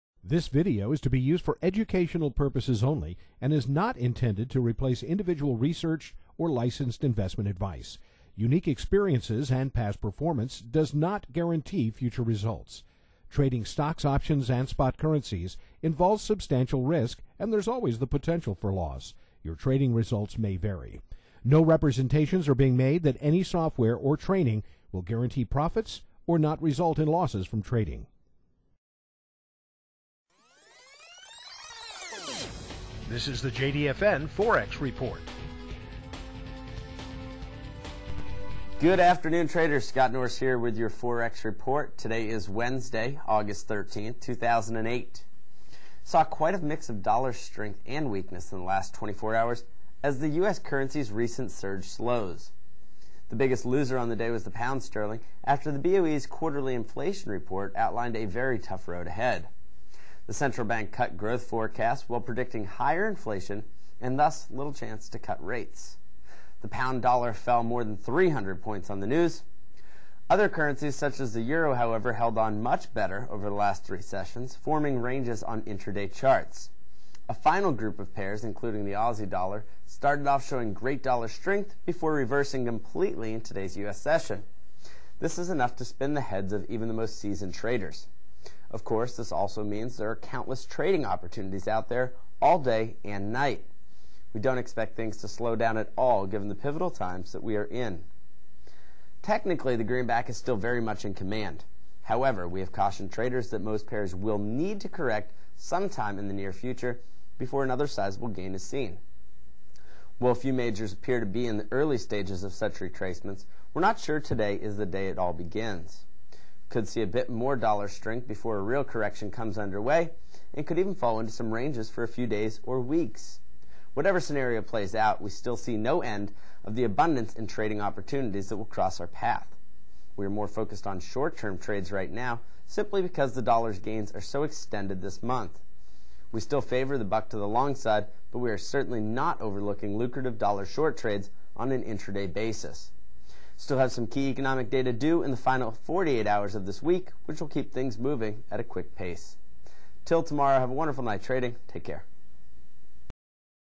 JDfn™ FOREX Report - 08/13/08
Join the James Dicks Financial Network's senior traders as they give a detailed report of the day's market activity

as well as upcoming events and possible reactions.